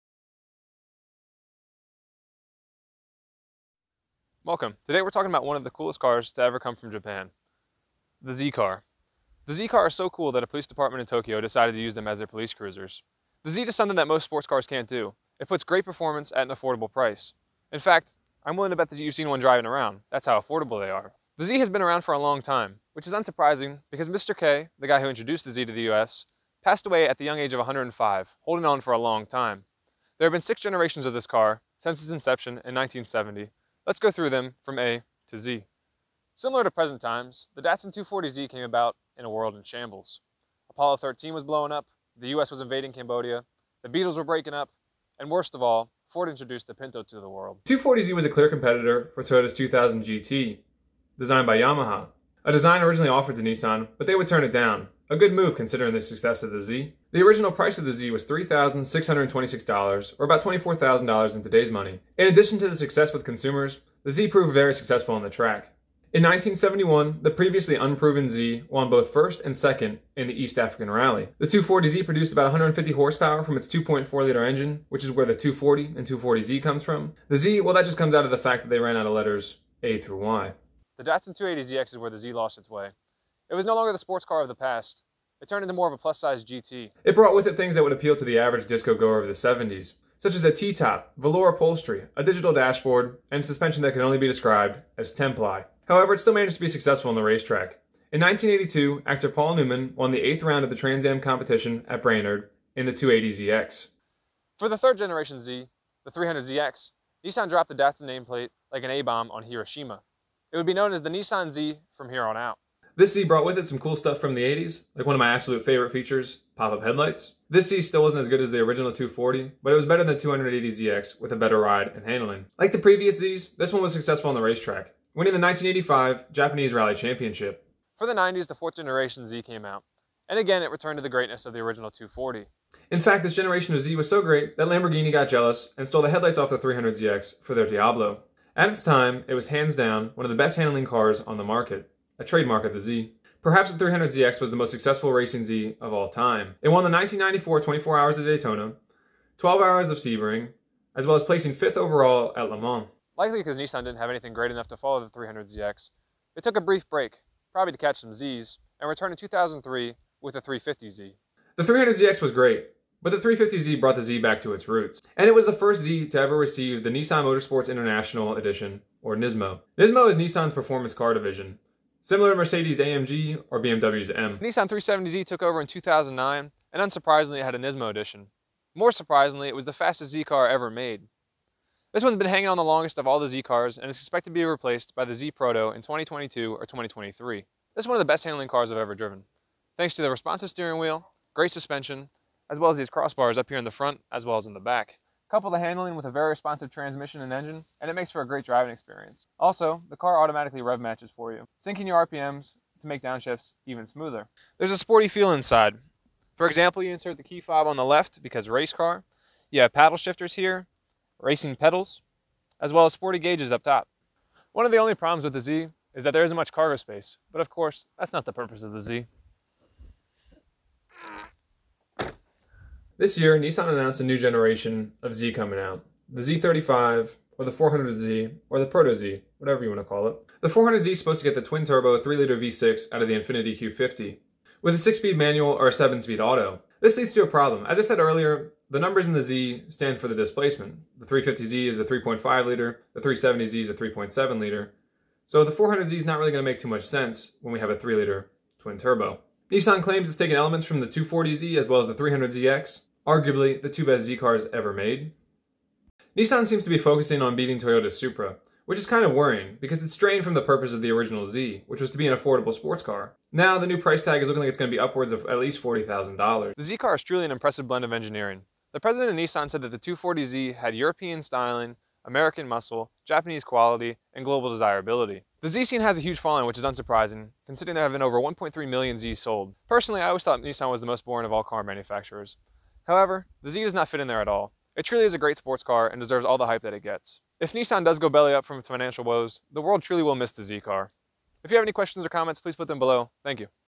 The Z-Car in Less Than 5 Minutes
This is a brief video about Nissan's Z-car From the 240Z to the 400Z, which is yet to be released.